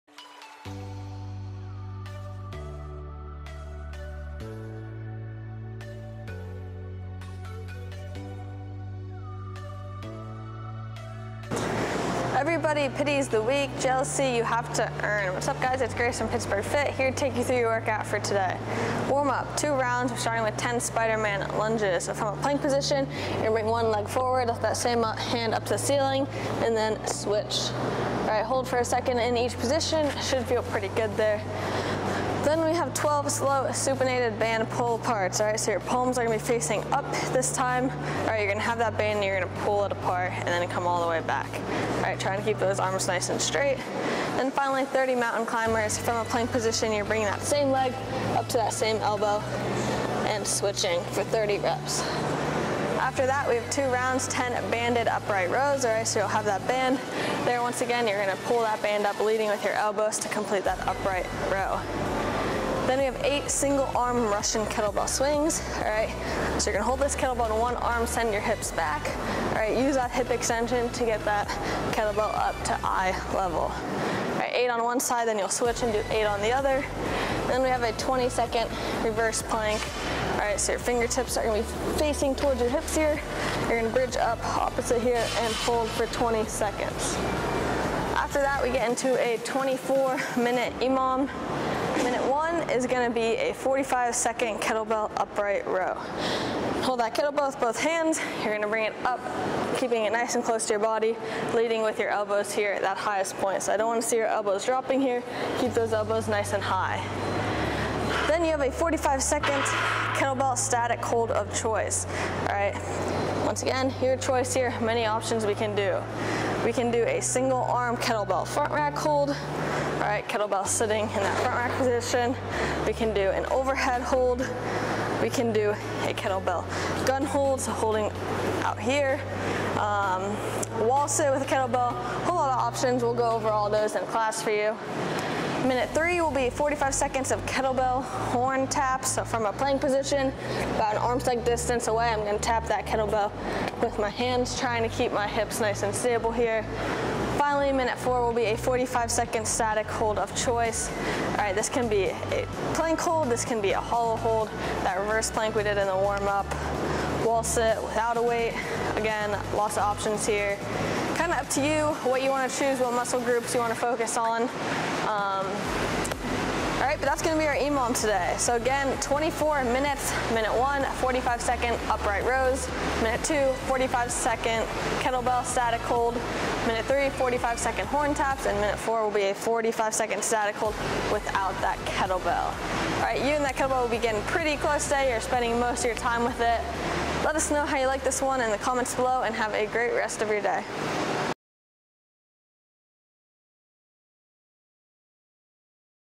PF Go Workout 110 4
"Everybody pities the weak; jealousy you have to earn."- Arnold Schwarzenegger

WARM-UP

2 Rounds

10 Spiderman Lunges

12 Slow Supinated Band Pull-Aparts

30 Mountain Climbers

2 Rounds

10 Banded Upright Row

8/8 Single Arm Russian Kettlebell Swing

:20 Reverse Plank

WORKOUT

EMOM x 24 MINUTES

MIN 1 - :45 KB Upright Row (Athlete Choice, Moderate)

MIN 2 - :45 KB Static Hold of Choice*

MIN 3 - :45 KB Horn Taps KB

MIN 4 - :45 Static Hold of Choice*

*Static Hold Options:

Single Arm KB Front Rack/ OH Hold

KB Gun Hold

Wall Sit (KB Weighted Optional)

Tall Plank

Tuck/ Hollow Hold

Reverse Plank

*Must choose 2 different Static Holds.